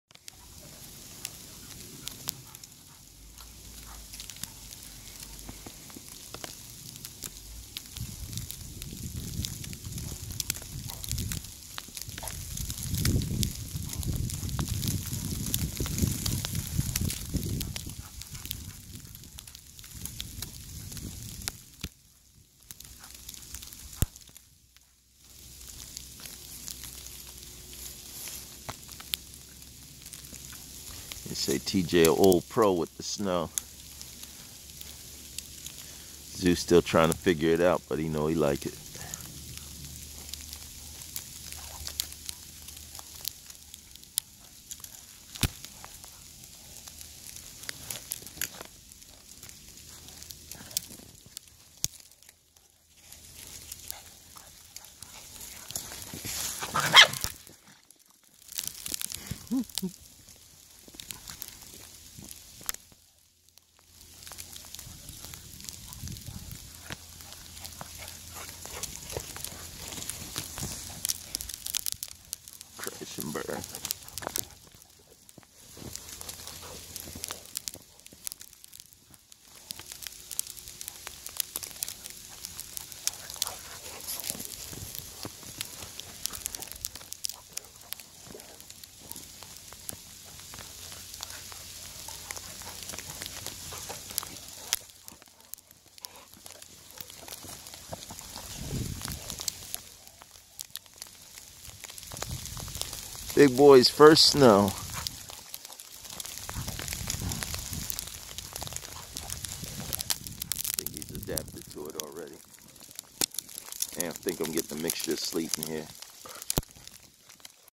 Zeus first snow day part 2
Dogs playing in the snow, English Mastiff puppy, boxer male, Teddy playing in the snow 2018